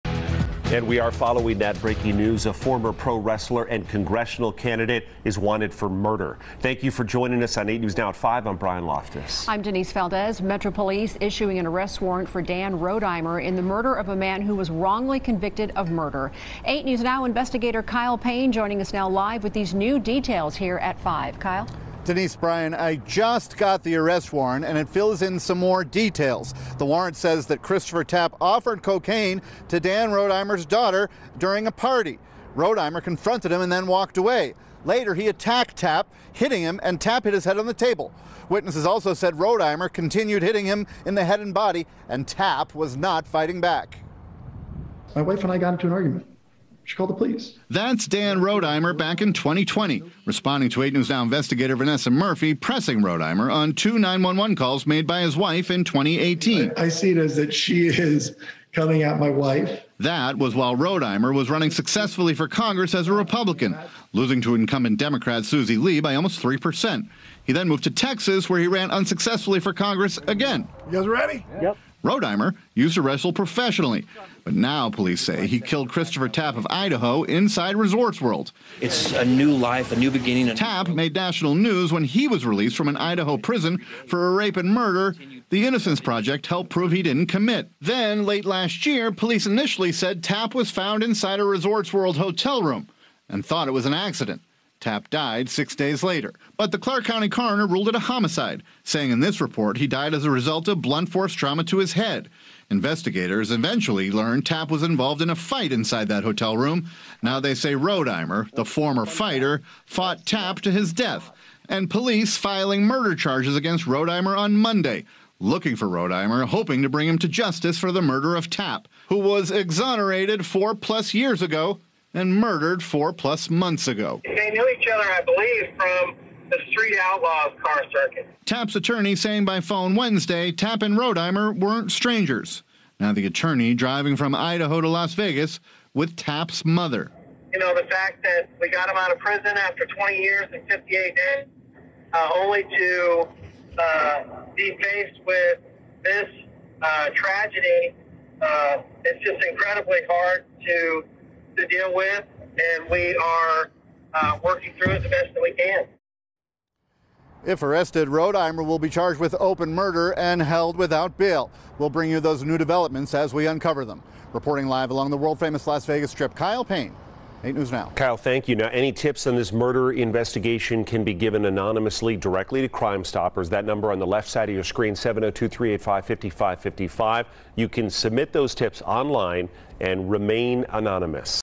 Ex-congressional candidate wanted in Las Vegas Strip murder
Las Vegas police detectives believe former Republican congressional candidate Dan Rodimer attacked a man, killing him, after the man — who himself was acquitted of murder — allegedly offered Rodimer’s stepdaughter cocaine during a party in a Las Vegas Strip hotel room.